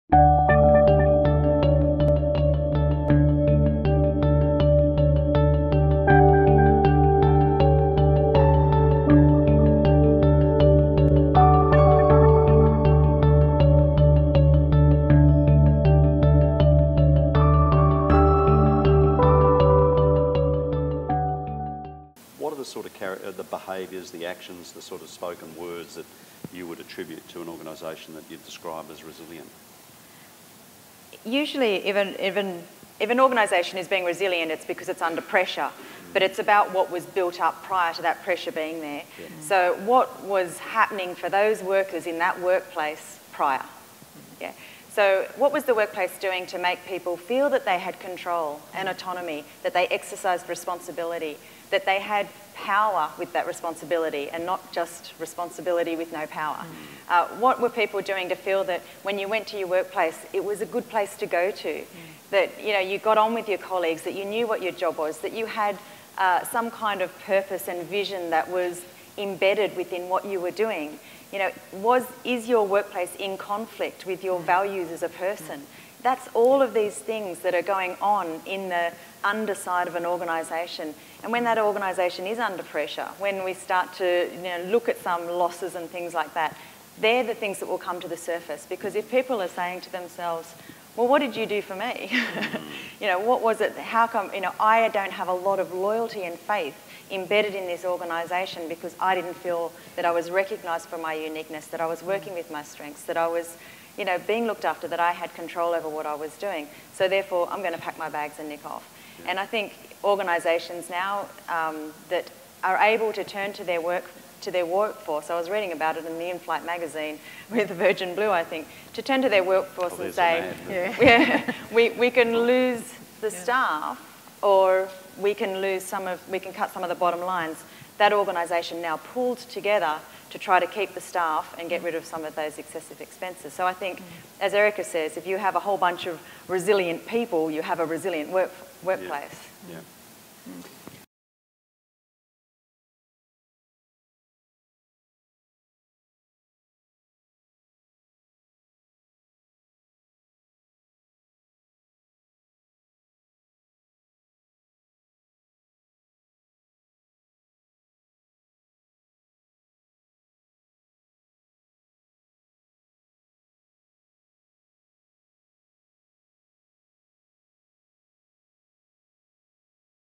Mental Health at Work 2010 Hypothetical - Ingrid Poulson May 2010
Speaking recently at MH@Work's inaugural conference, "Mental Healthiness, Adversity and Resilience at Work".